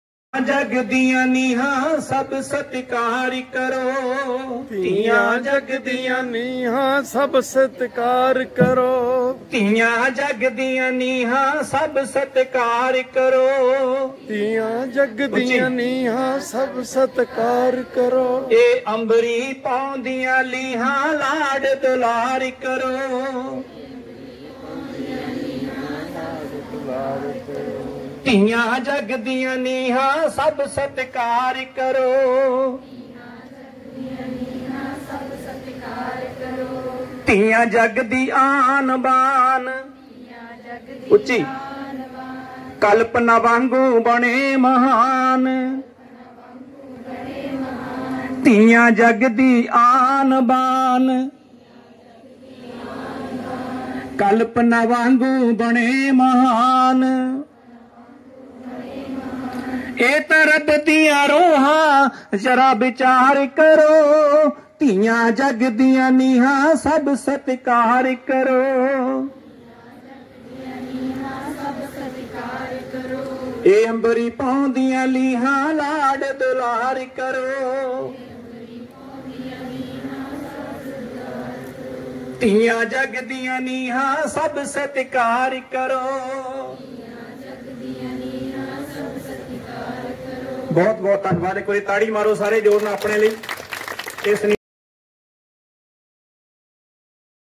ਧੀਆਂ ਜੱਗ ਦੀਆਂ ਨੀਹਾਂ
ਮਿਤੀ 4 ਅਕਤੂਬਰ 2023 ਦਿਨ ਬੁੱਧਵਾਰ, ਸਮਾਂ ਸਵੇਰ ਦੇ 10.00 ਵਜੇ ਲੜਕੀਆਂ ਦੇ ਸਰਕਾਰੀ ਆਈ.ਟੀ.ਆਈ ਨਵਾਂਸ਼ਹਿਰ ਵਿਖੇ ਜਾਗਰੂਕਤਾ ਸੈਮੀਨਾਰ ਹੋਇਆ।